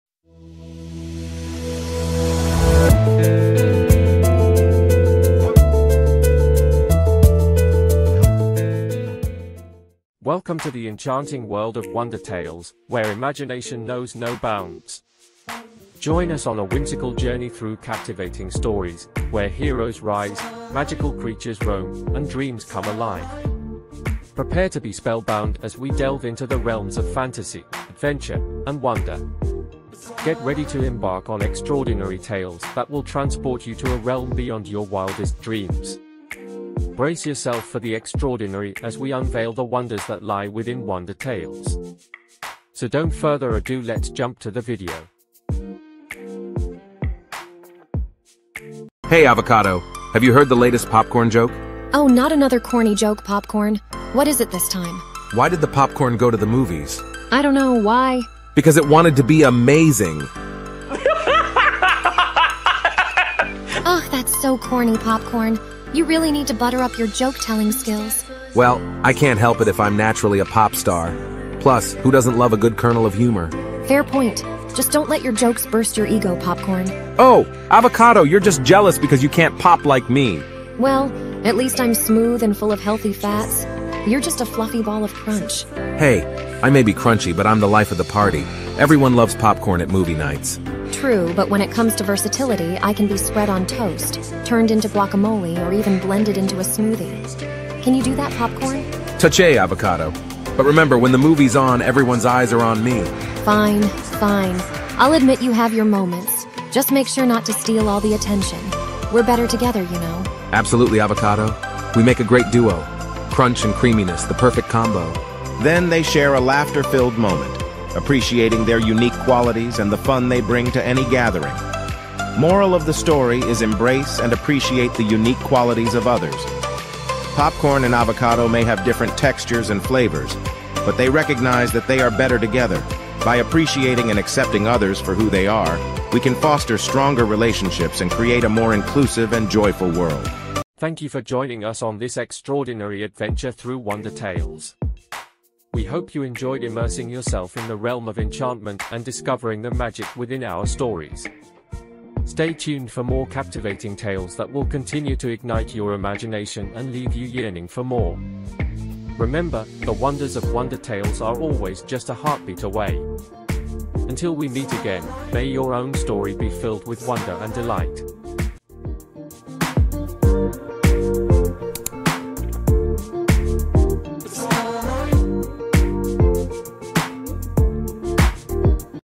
Popcorn & Avocado 🍿🥑😋| Magical short story | WonderTales
Popcorn and Avocado Delight" is an imaginative and heartwarming short story that will captivate readers of all ages. It seamlessly blends elements of fantasy, adventure, and self-discovery, leaving readers with a sense of wonder and a renewed appreciation for the extraordinary beauty that can be found in the most unexpected places.

Queries Solved
• Short stories.
• Animation.
•  Moral stories for kids.
• English Communication.
• Animated short stories.

Video made by :@WonderTales1713